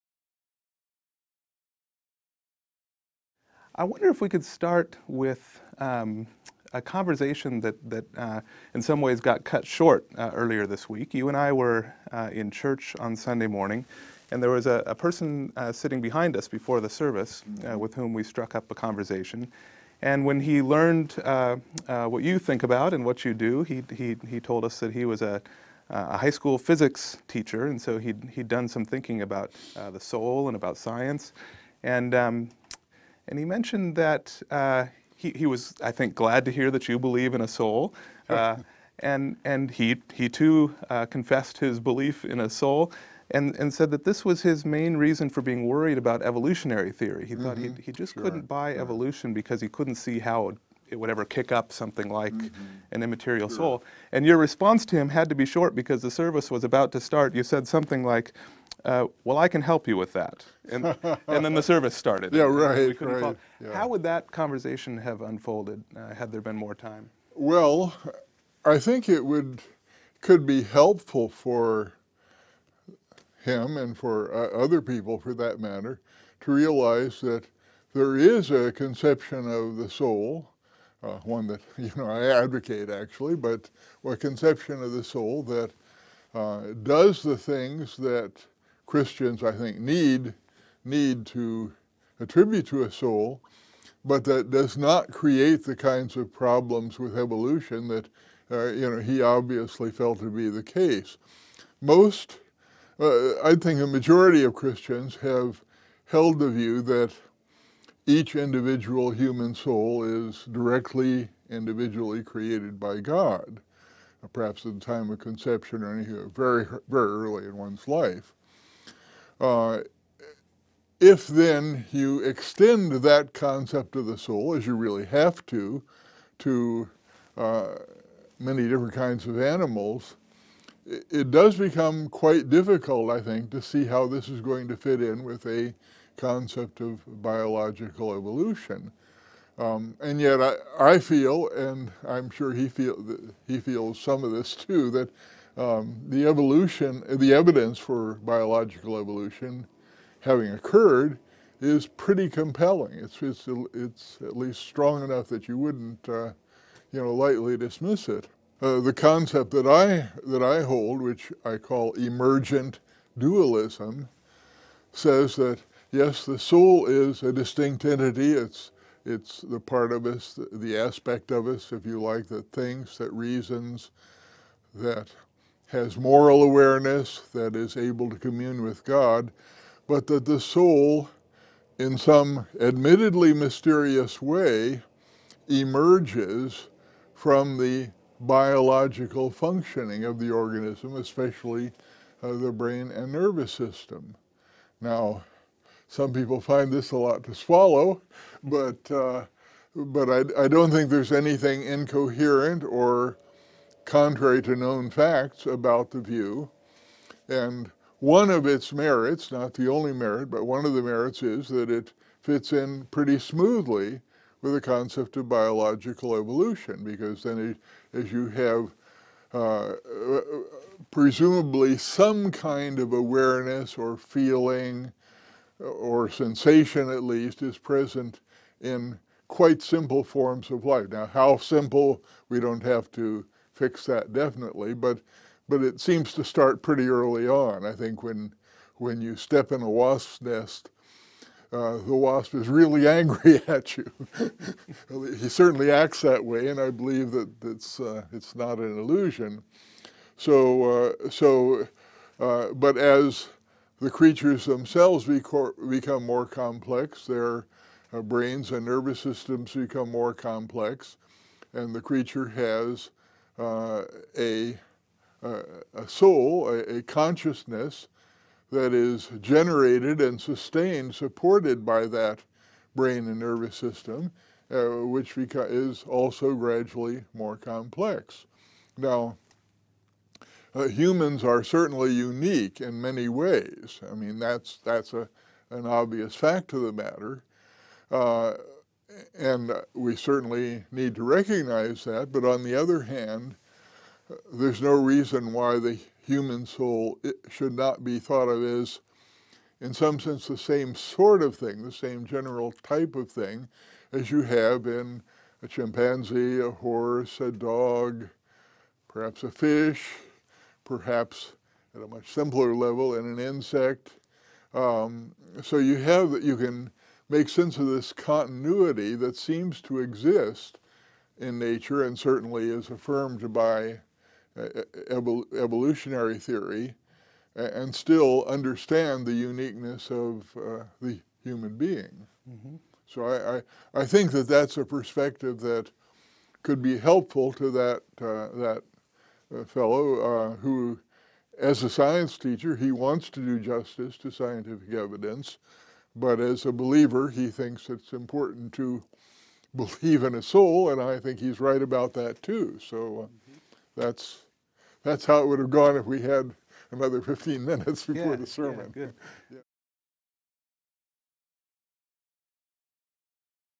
William Hasker: The Evolution of Souls
William Hasker (Huntington College) suggests a view of the human soul he claims is compatible with the science of evolution.